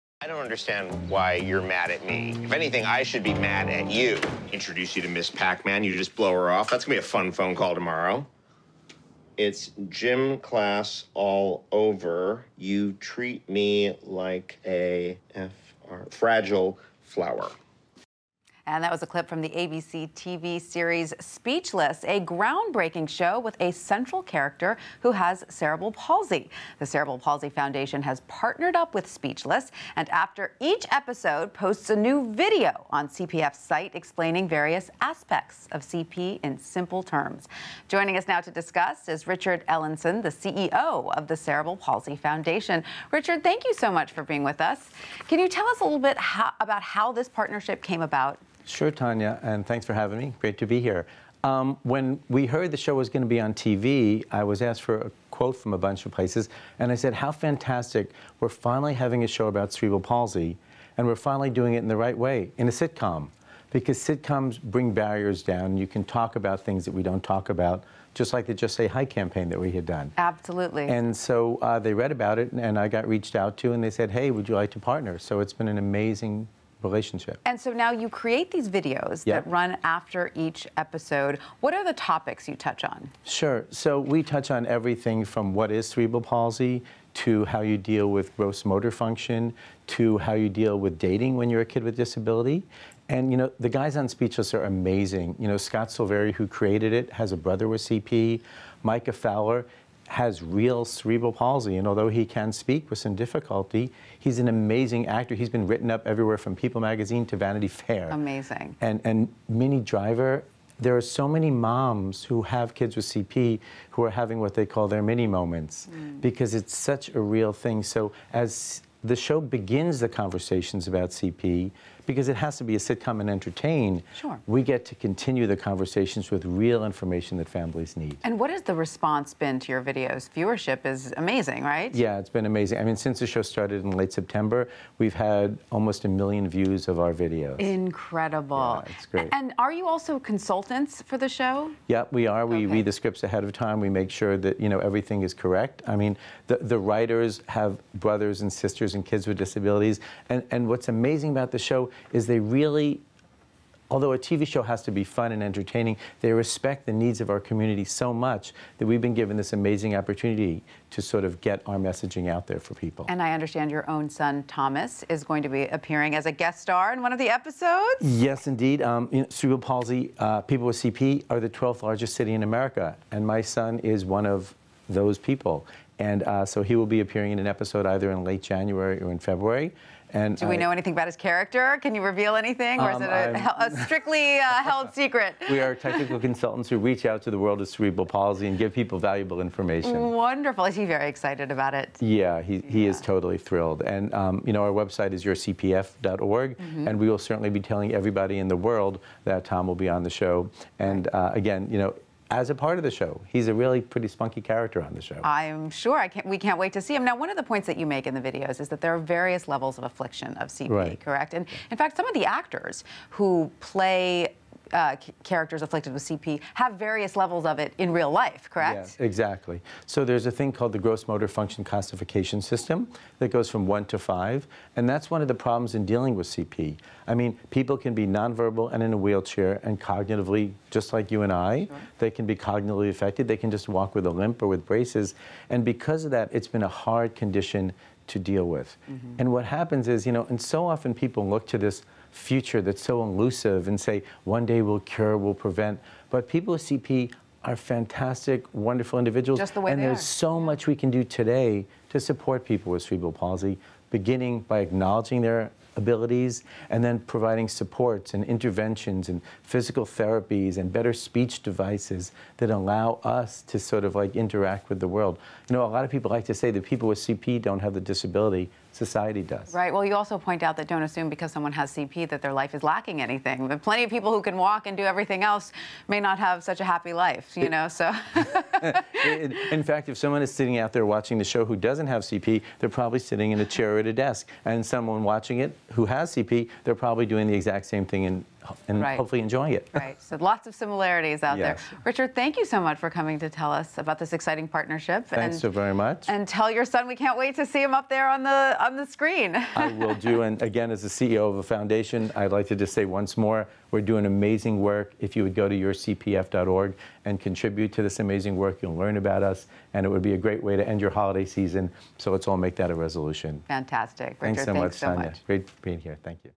Cerebral Palsy Foundation Teams Up With ABC's 'Speechless'
ABC's series "Speechless," featuring a teen living with cerebral palsy, has teamed up with the Cerebral Palsy Foundation to educate the public about the affliction. CPF CEO Richard Ellenson sits with Lunch Break's Tanya Rivero to discuss the group's partnership. Photo: Cerebral Palsy Foundation